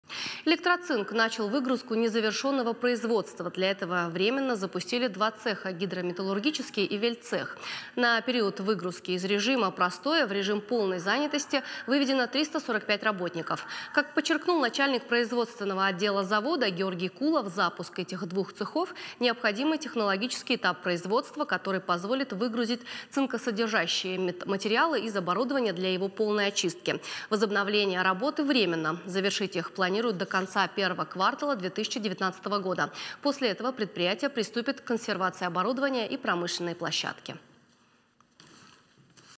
«Электроцинк» начал выгрузку незавершенного производства
«Электроцинк» начал выгрузку незавершенного производства. Для этого временно запустили два цеха: гидрометаллургический и вельццех. На период выгрузки из режима простоя в режим полной занятости выведено 345 работников.

Как подчеркнул начальник производственного отдела завода, Георгий Кулов, запуск этих двух цехов - необходимый технологический этап производства, который позволит выгрузить цинксодержащие материалы из оборудования для его полной очистки. Возобновление работы временно.

Завершить их планируют до конца первого квАртала 2019 года. После этого предприятие приступит к консервации оборудования и промышленной площадки.
Мы рядом в:
ok.ru/irystontv